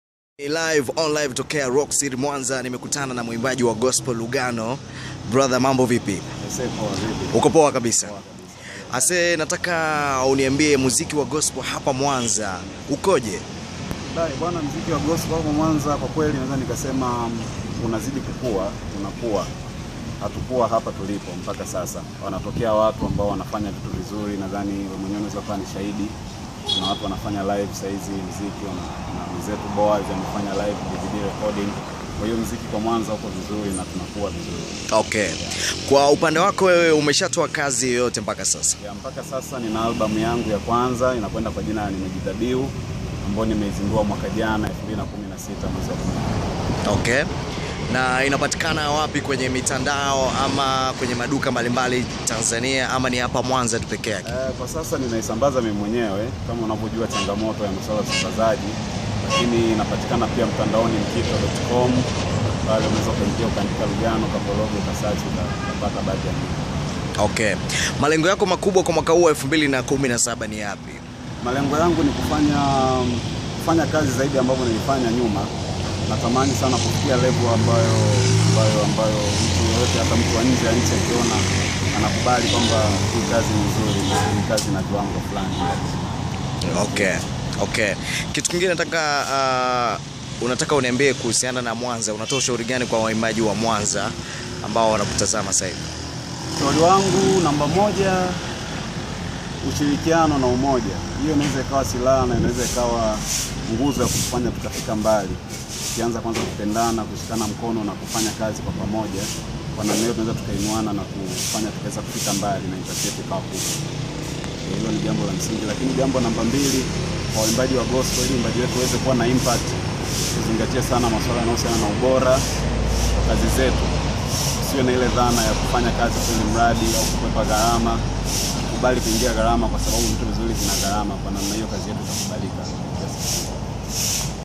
Lugano Anasema Muziki Wa Gospel Mwanza Unaendelea Kukua Kila Siku
Lugano Ni Muimbaji Wa Gospel Kutoka Rock City Mwanza Anafunguka Mambo Kadhaa Kuhusu Muziki Wake.